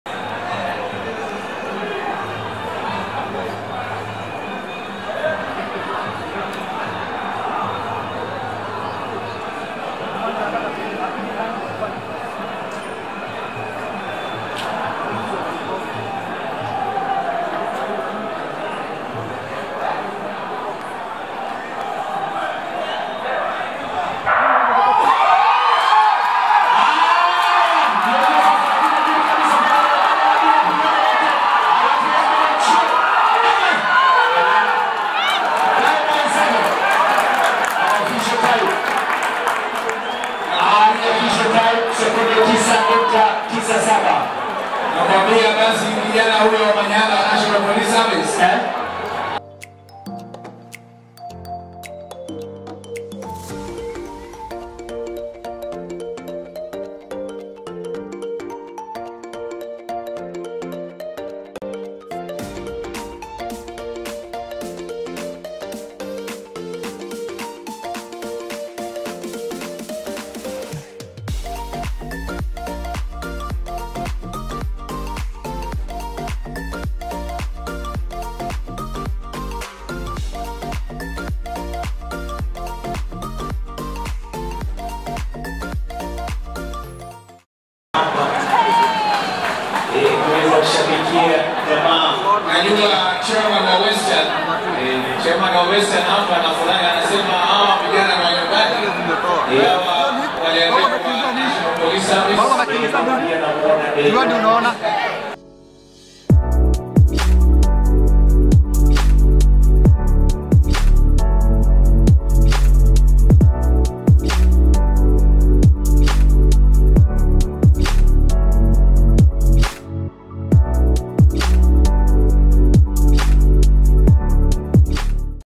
Omanyala clocks 9.97 seconds at Nyayo Stadium | MUST SEE
Omanyala ran 9.96 seconds during the 2023 Kenyal National Championships held at Nyayo Stadium